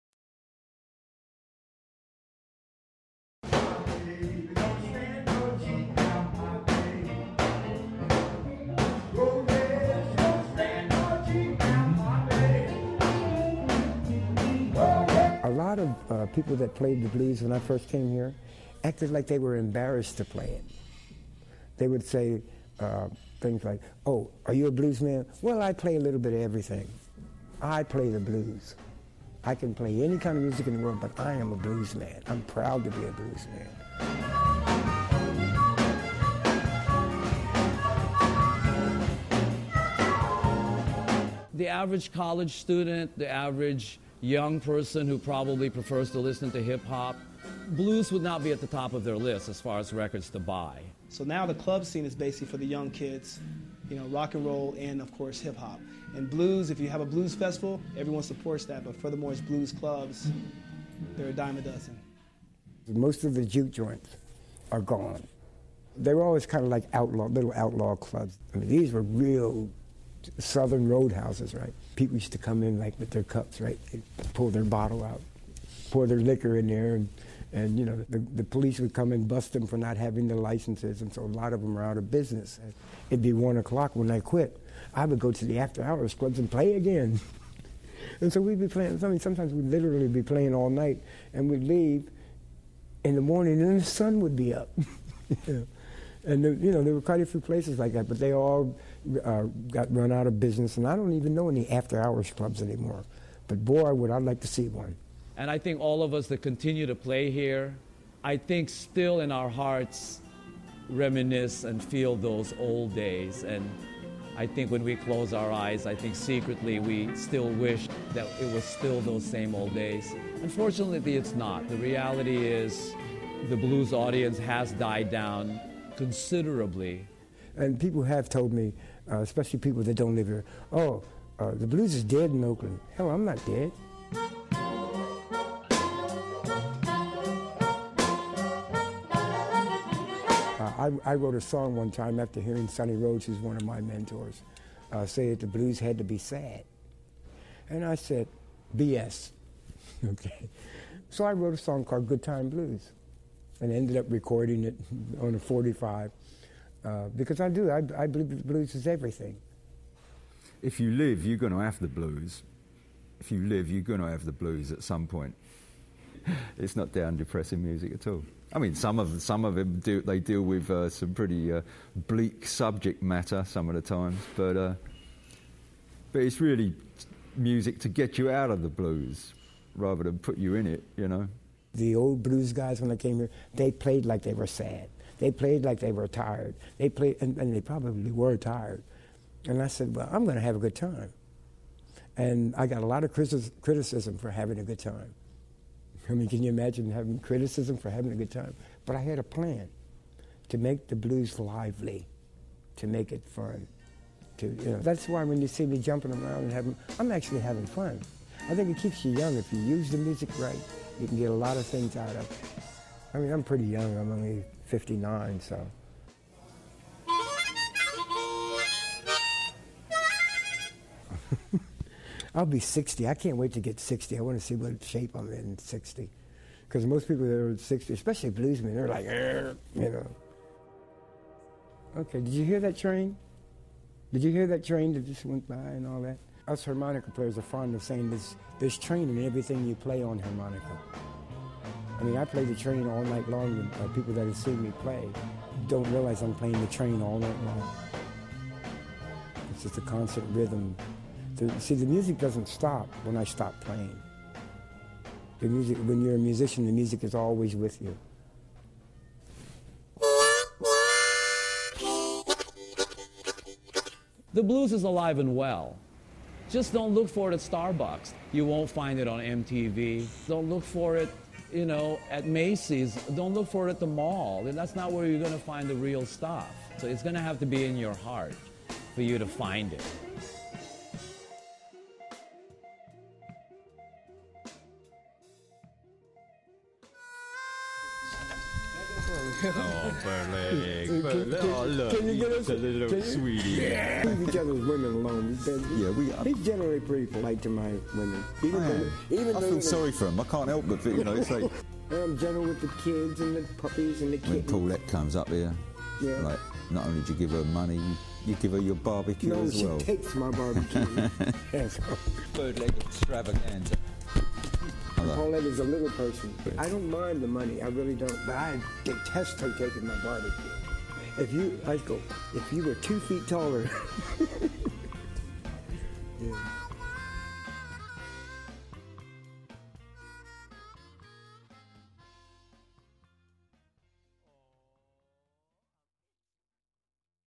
Birdleg and Friends
Oakland Ca. Blues Documentary
Producer: Thieny Hoang
On set and Post Audio by: Thomas Patrick and Dan Hoyt